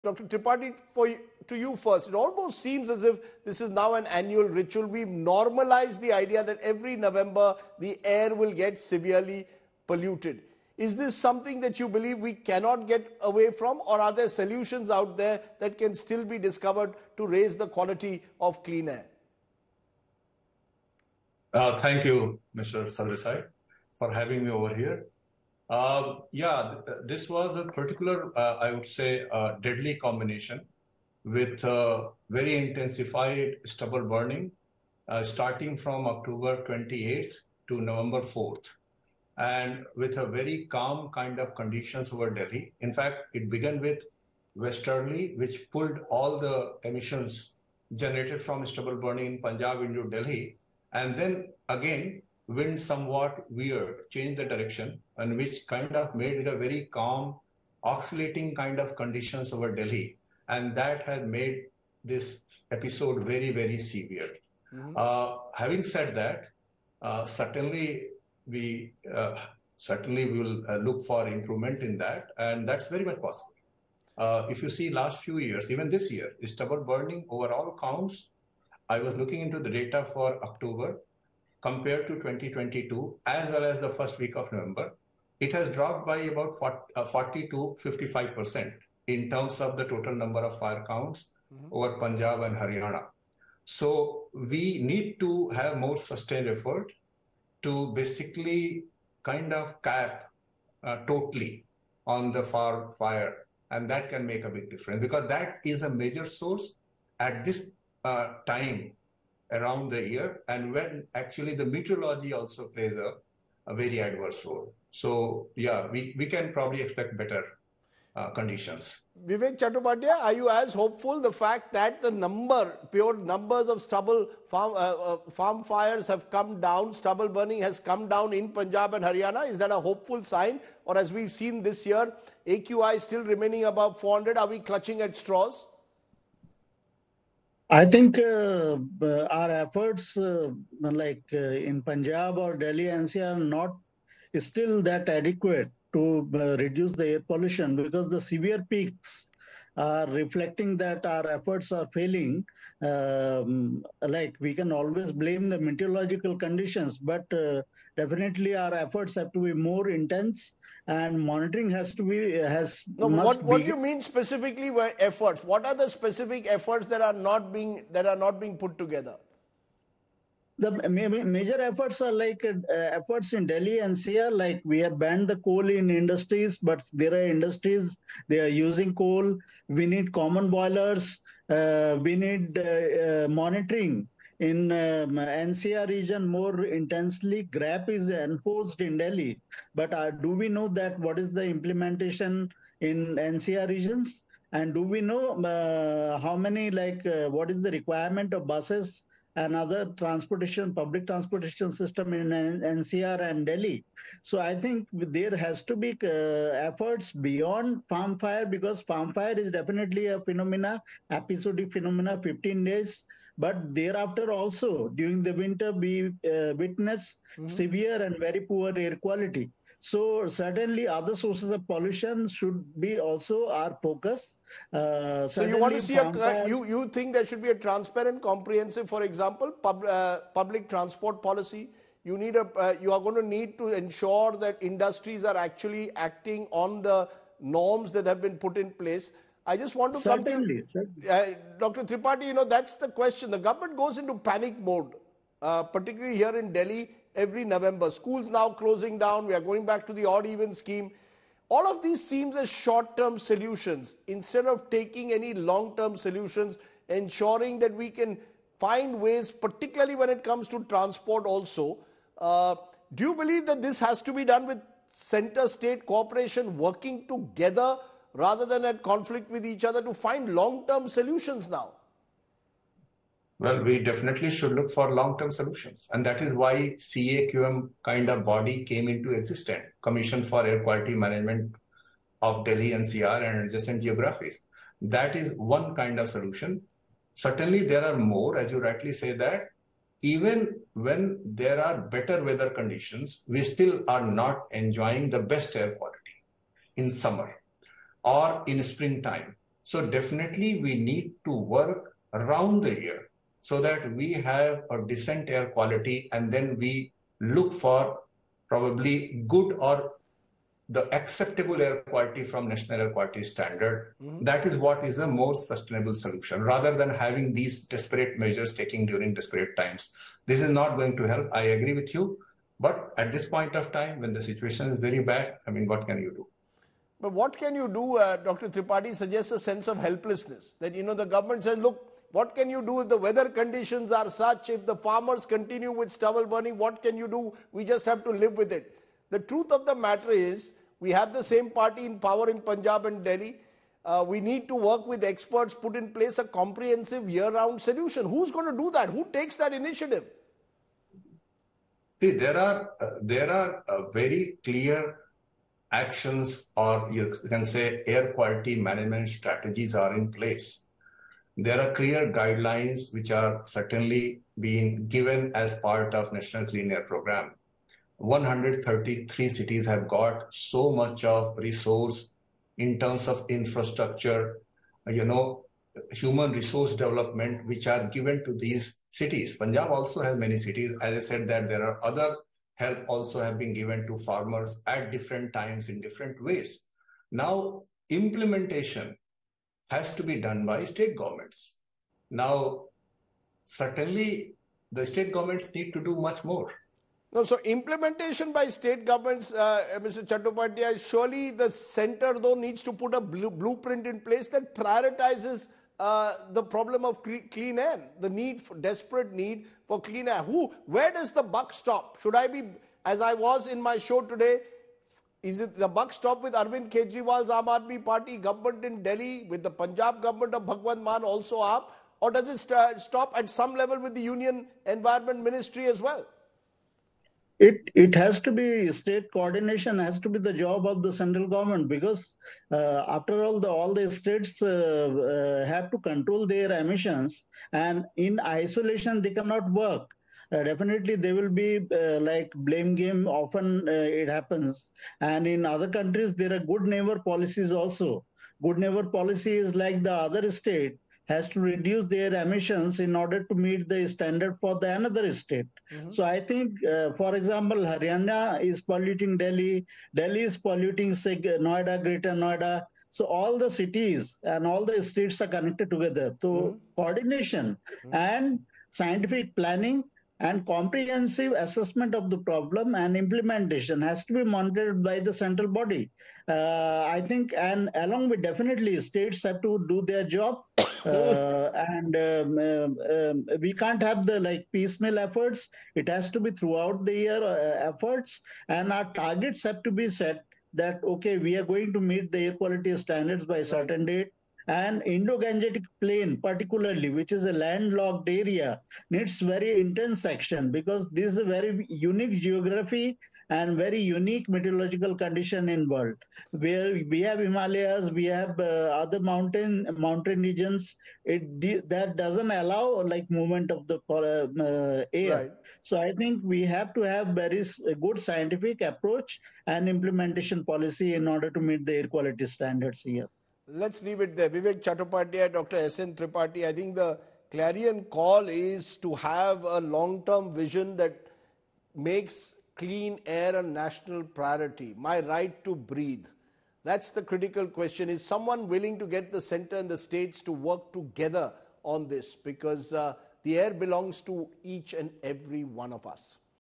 Have We Normalised Idea Of Delhi Air Pollution? Dr. Sn Tripathi & Vivek Chattopadhyaya Reponds
Delhi continues to be covered in thick haze as the air quality remains in the 'severe' category, data from the Central Pollution Control Board (CPCB) showed on Monday. This is the fourth consecutive day that the national capital recorded a 'severe' air quality. The overall air quality index (AQI) of Delhi was recorded at 437 at 9 am, according to CPCB data.